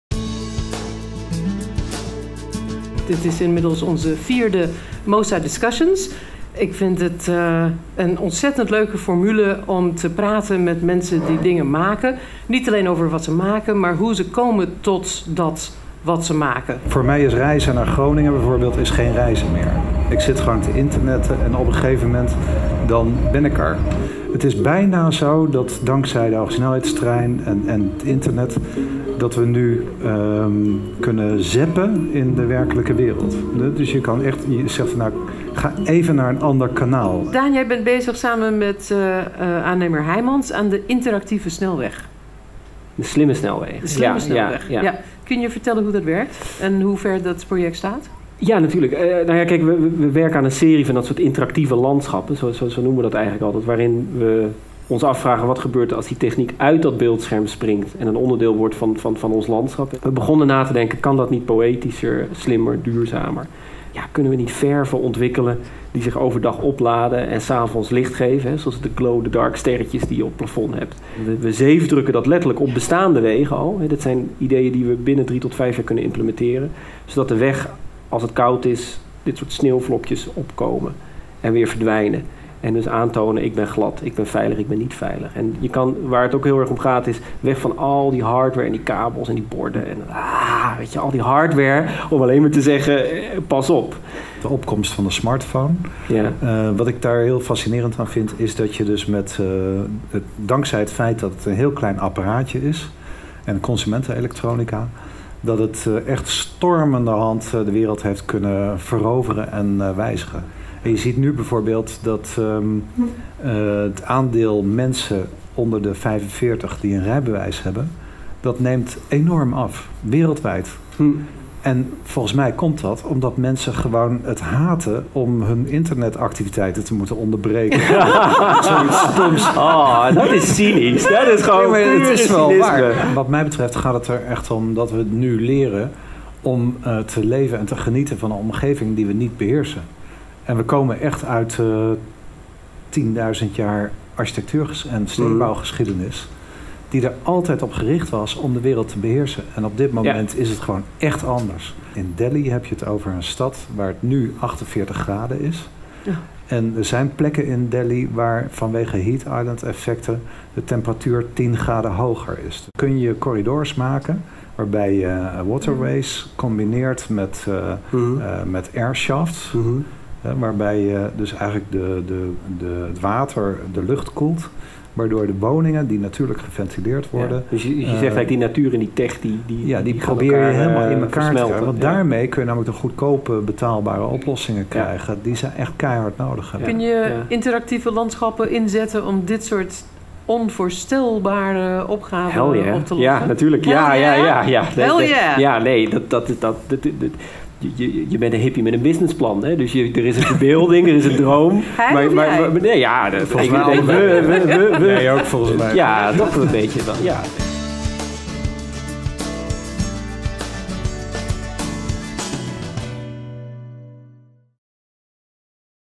Mosa Discussions #4: Sensations
Onder leiding van Tracy Metz (NRC Handelsblad) spreken kunstenaar Daan Roosegaarde (StudioRoosegaarde) en Ton Venhoeven (Venhoeven CS architecture+urbanism) aan de hand van een serie (film)beelden over hun persoonlijke inspiratie en drijfveren. Tijdens het gesprek is er ruimte voor interactie tussen publiek en sprekers. Het overkoepelend thema van de avond is 'Sensations'. Dit rondetafelgesprek is het vierde in de reeks Mosa Discussions.